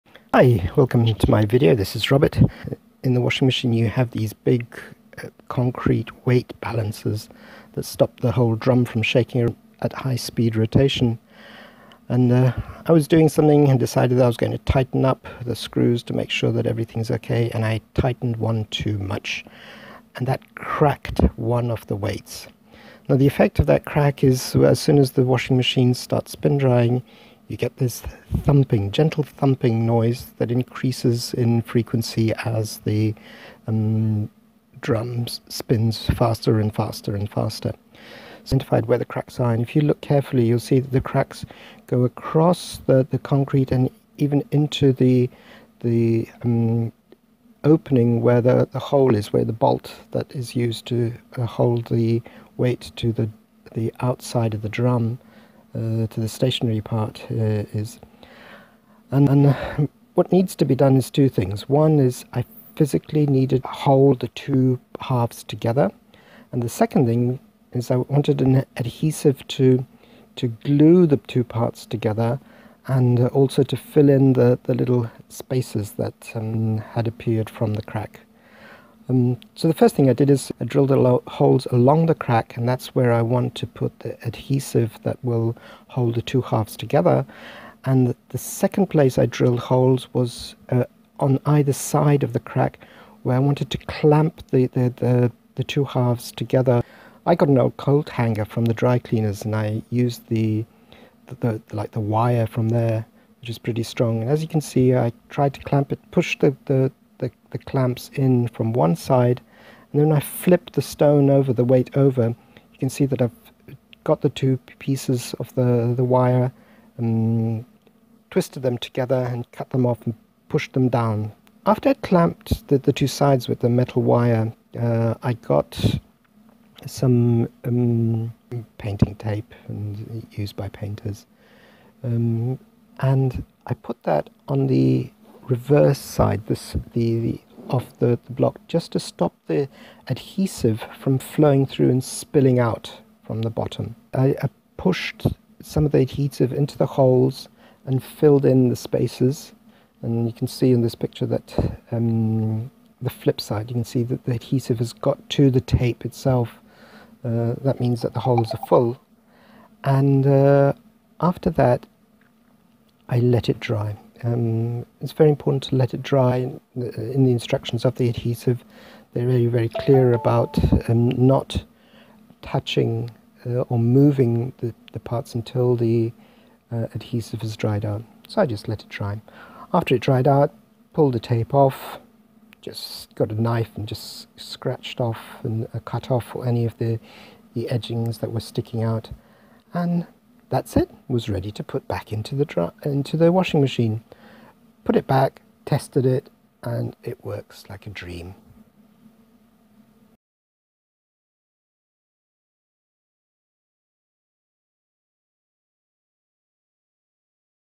How to fix a washing machine balance weight to stop noise when spin drying
Is your washing machine making a knocking noise or a thumping noise? Is your washing machine moving around the washing room? Do you have dust underneath your washing machine?

You probably have a cracked balance weight in your washing machine. How do you fix a cracked balance weight? Well this video shows you how to fix a cracked balance weight. If the balance weight is broken - i.e. one part has completely broken off and fallen onto the bottom of the washing machine, then it is better to buy a new one.

The dust underneath your washing machine may be coming from the cracked balance weight movements at the crack. Open the washing machine and have a look inside. If you need a new balance weight try espares.co.uk . If you need to learn how to change a balance weight, espares has an excellent tutorial